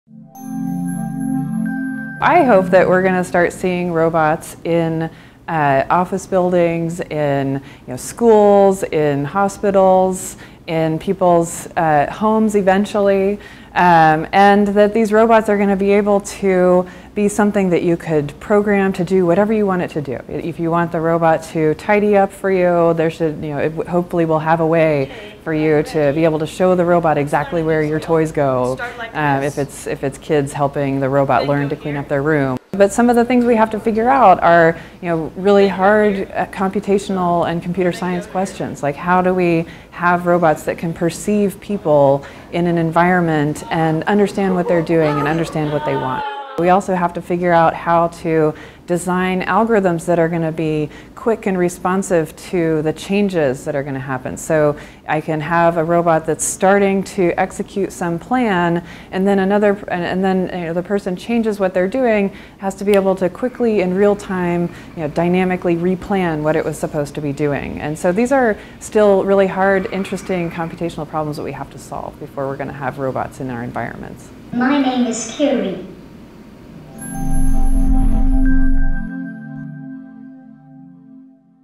National Robotics Week - Andrea Thomaz
Georgia Tech Associate Professor Andrea Thomaz studies human-robot interaction. She discusses the a future of robots and humans interacting together in homes, offices and elsewhere in the "real world."